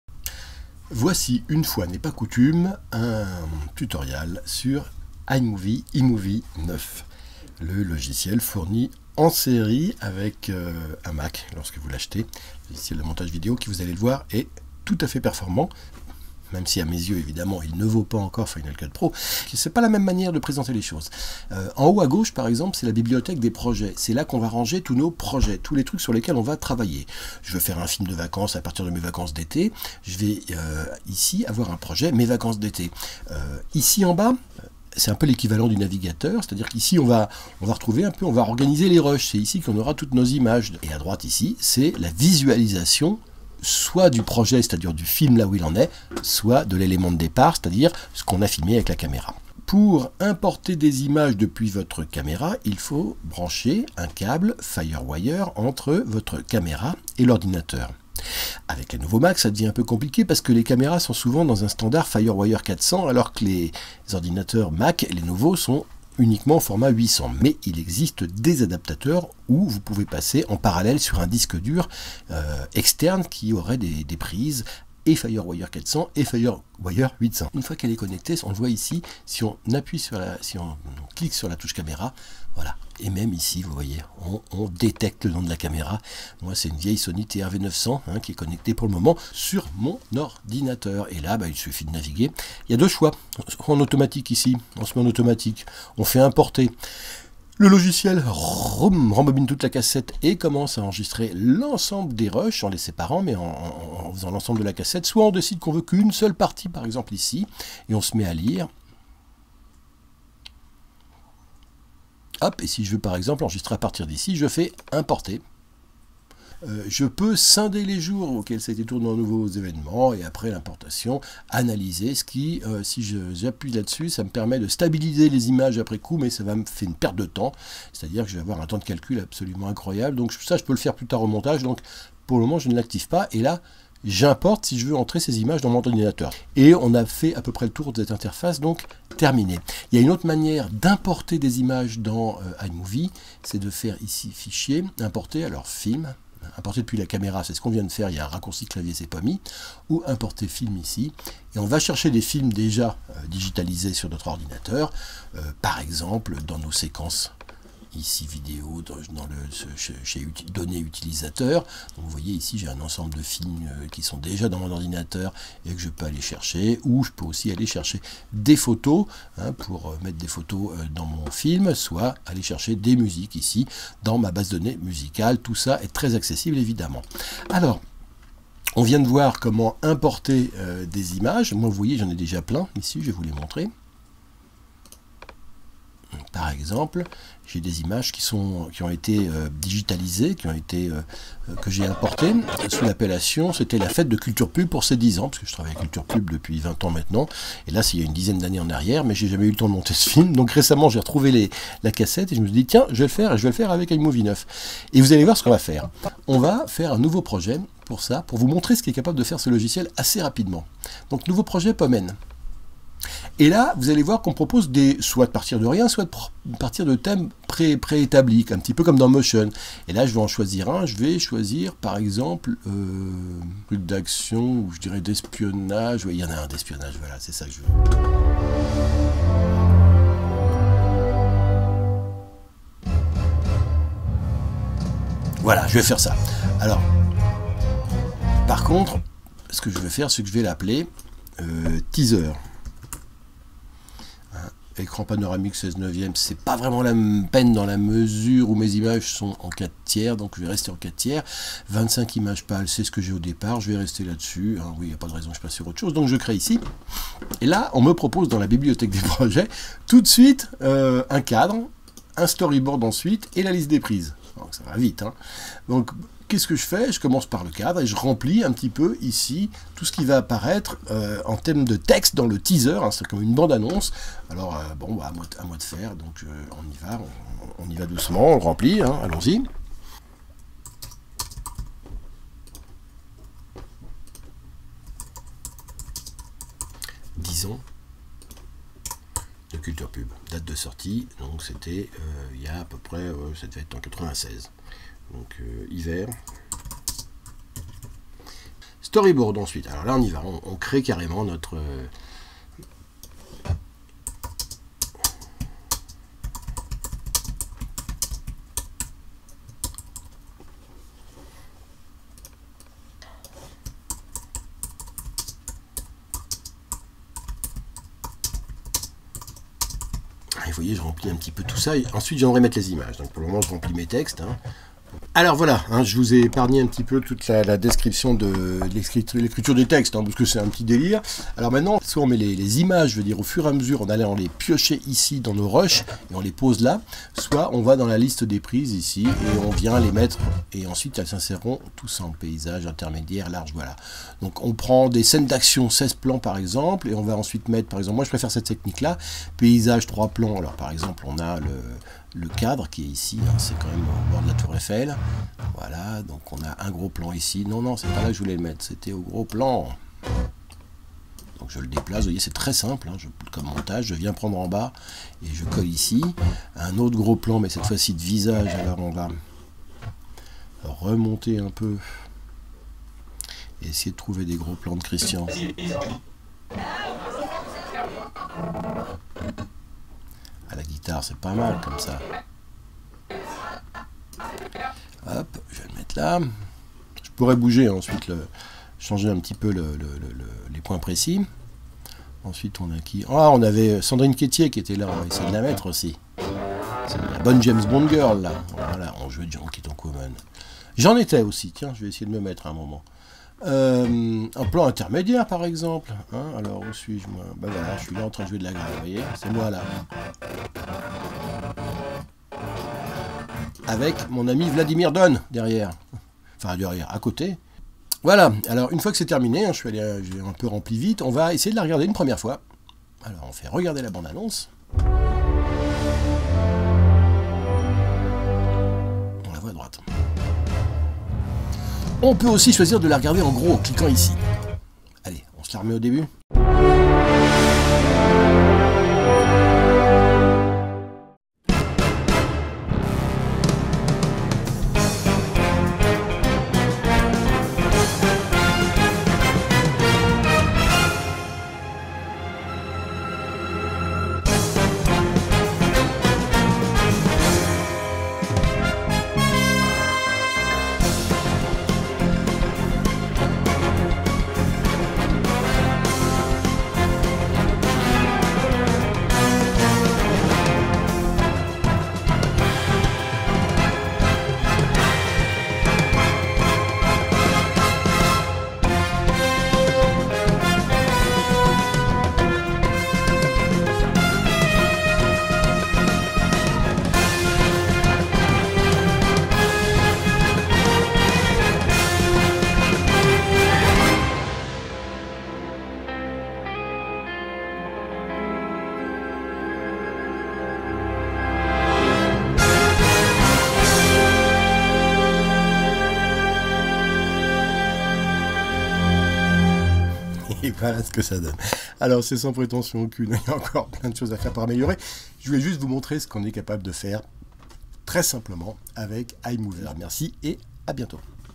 Faire une bande-annonce sur iMovie9
tutorial video sur la première prise en main d'iMovie9 et déjà, comment réaliser simplement une bande-annonce à partir de quelques images de films de vacances. Avec Severine Ferrer, Sandrine Quettier, Naguy, PPDA, Paul Persone...